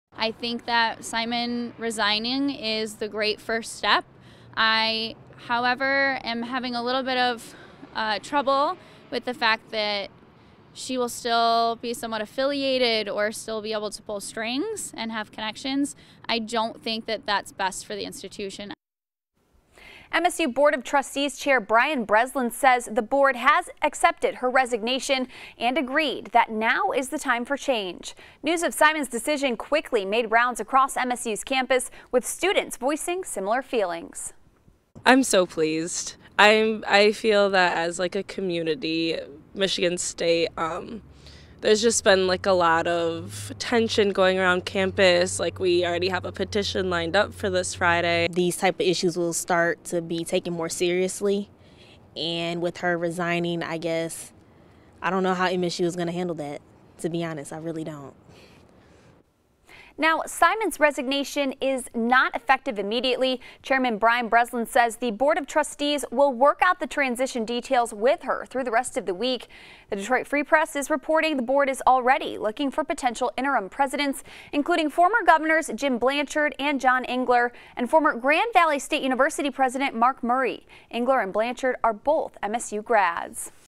Spartans react to Simon's resignation
Some Michigan State University students were already planning a protest for Friday to call for Lou Anna K. Simon to resign.